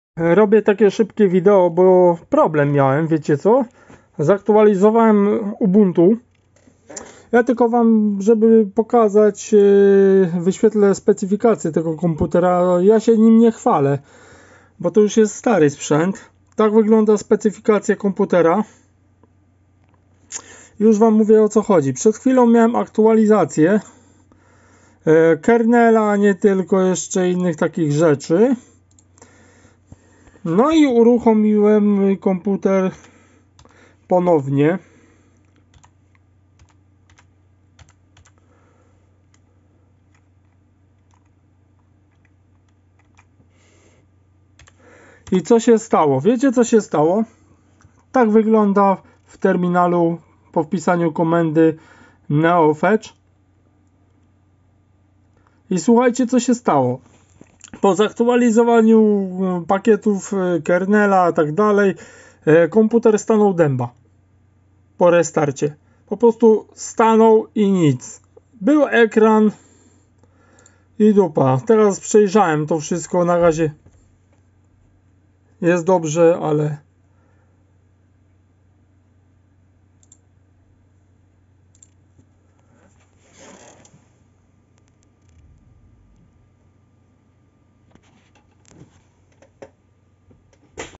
Mega Usterka
Po aktualizacji pakietów i restarcie komputera wszystko stanęło w miejscu. dopiero pomogło odłączenie od prądu na pięć minut.